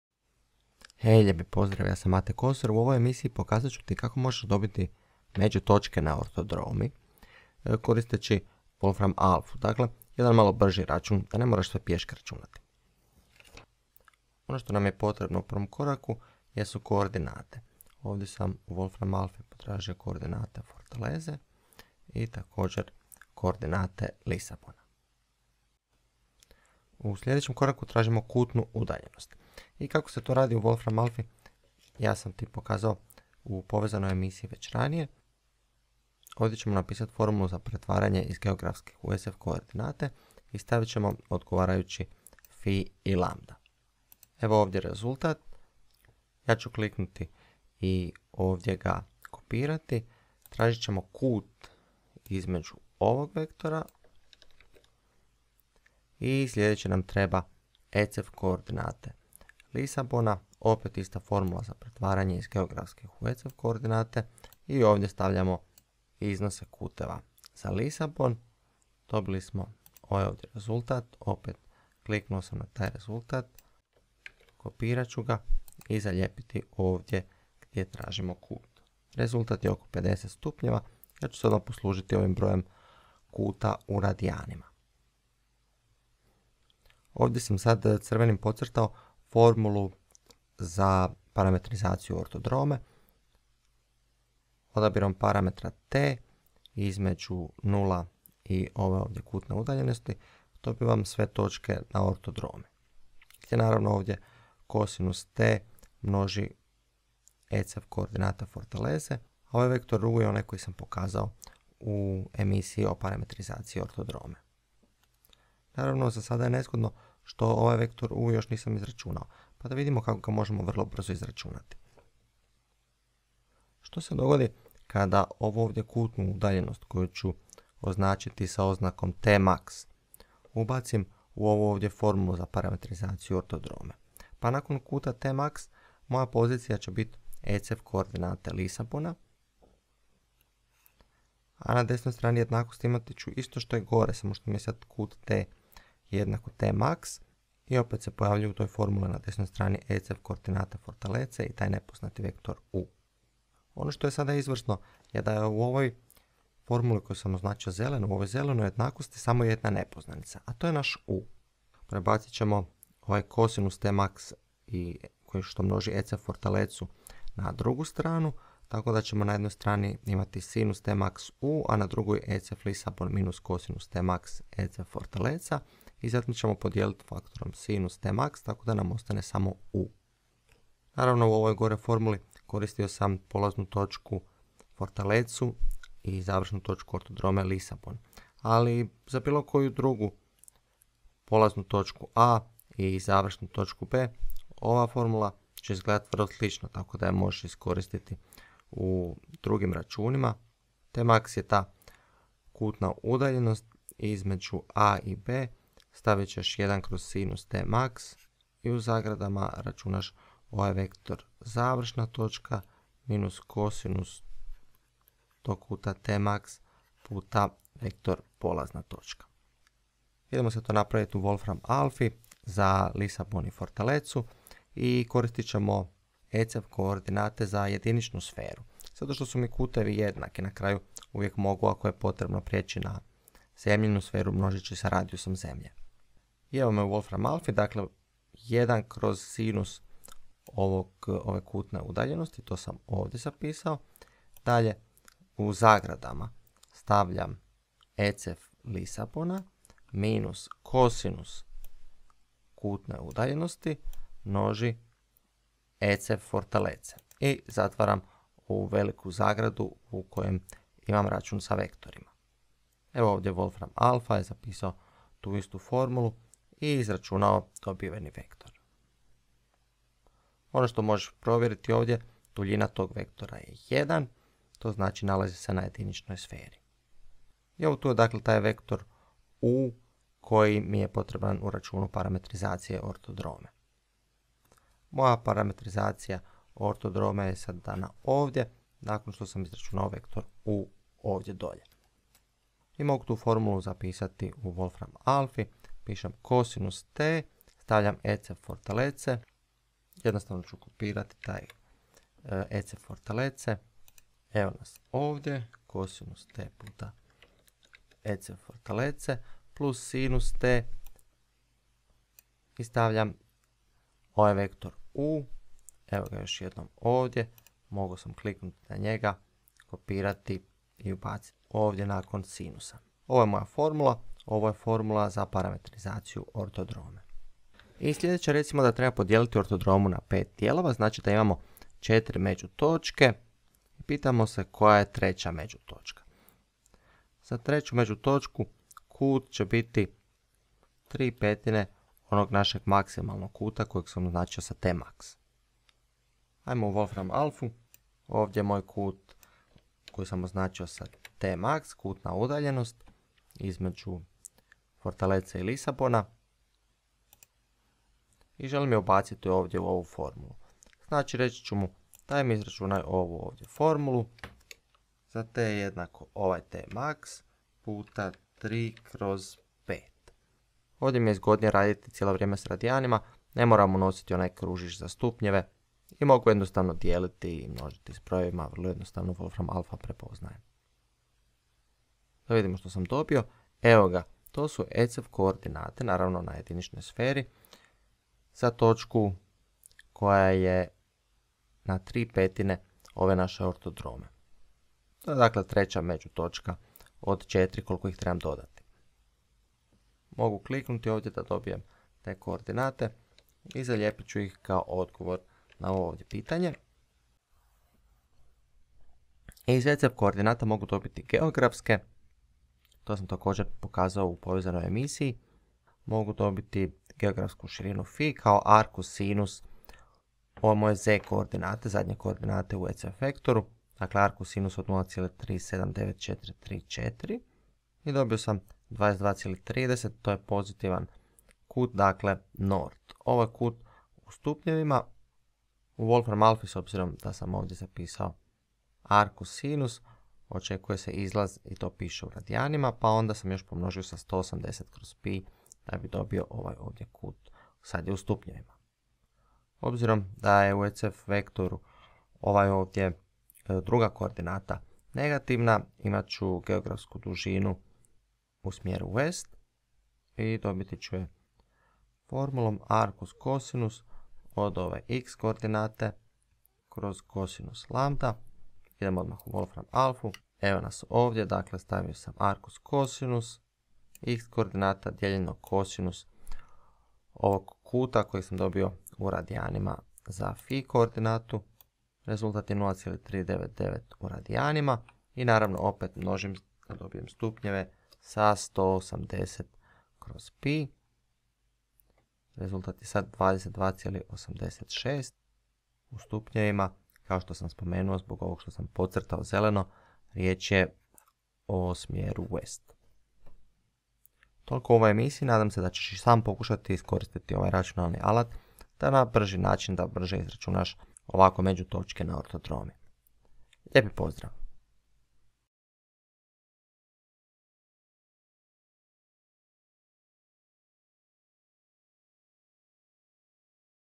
Međutočke na ortodromi - brže koristeći WolframAlpha
U povezanoj emisiji pokazao sam kako pronaći parametrizaciju ortodrome i međutočke. Ovdje isto radim na nešto brži način koristeći računalni alat WolframAlpha.
7:47 Greška! U nazivniku formule umjesto cos (lambda) treba biti cos(phi). Dalje je ispravno uvršteno...